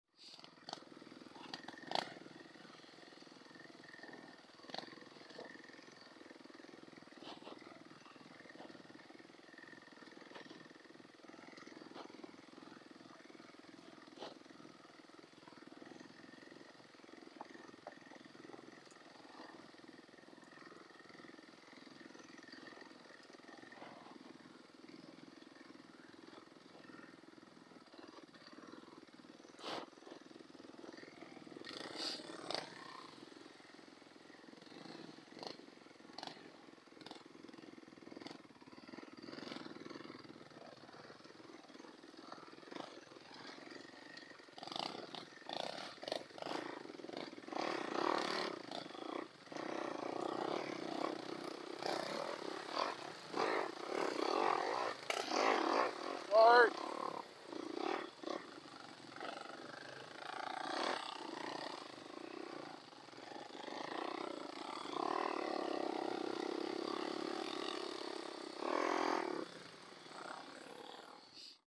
Rock Run Clip
This is a little clip from Rock Run Recreation Park. I thought it was neat watching the guys on quads go over the rocks. I kinda feel like I cut that one guy off. Anyone on trail etiquette in the comments?